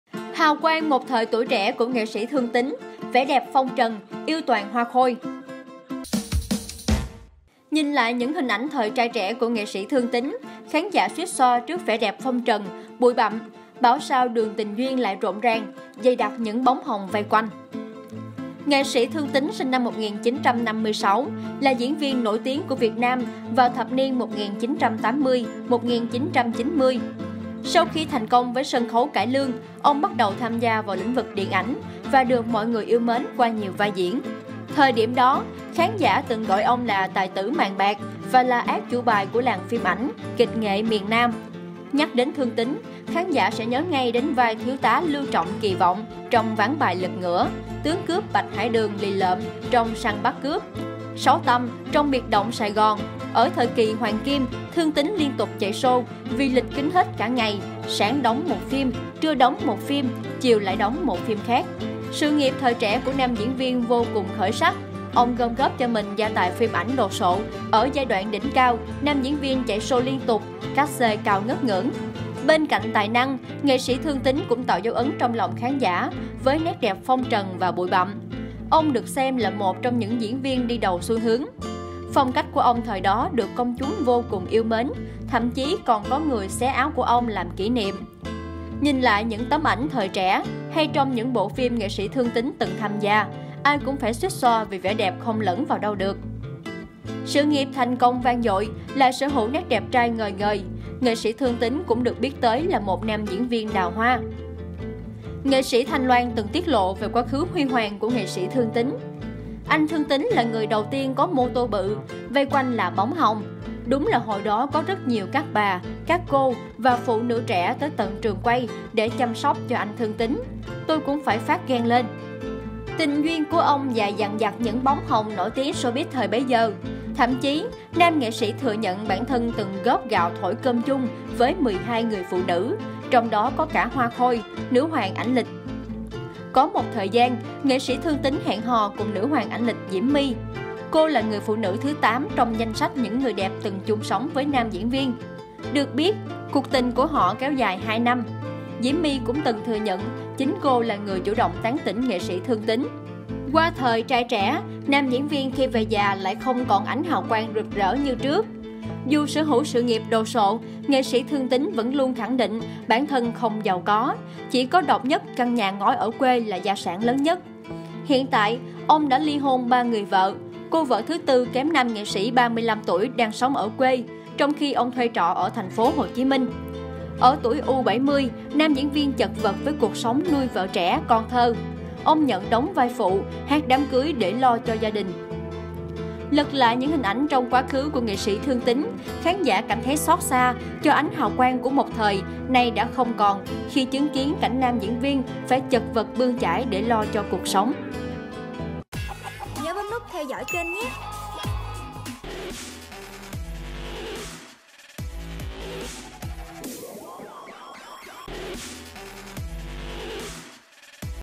Hào quang một thời tuổi trẻ của NS Thương Tín: Vẻ đẹp phong trần, yêu toàn Hoa khôi
Hãy theo dõi Yan trên:
✔️Youtube Yan News :